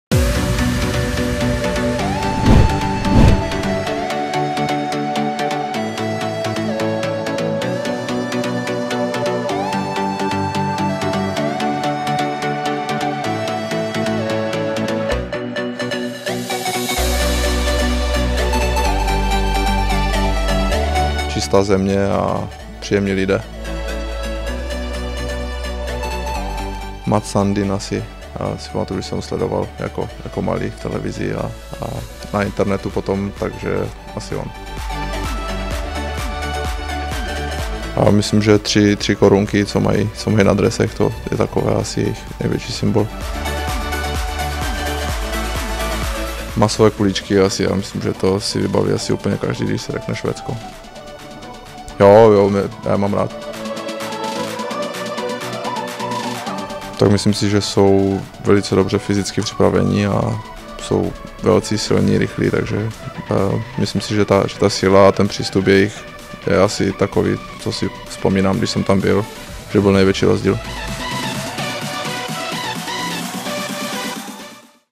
Švédsko: Tradičního soupeře představuje Matěj Stránský
Švédsko patří mezi nejčastější protivníky českého národního týmu. Pro české hokejisty je to na letošním MS v Rize čtvrtý soupeř. Seveřané zatím na turnaji překvapili hodně matnými výkony. Během své kariéry si ve Švédsku zahrál i útočník Matěj Stránský. V čem jsou tedy podle něj Švédové na ledě nebezpeční, jak charakterizoval tuto skandinávskou zemi?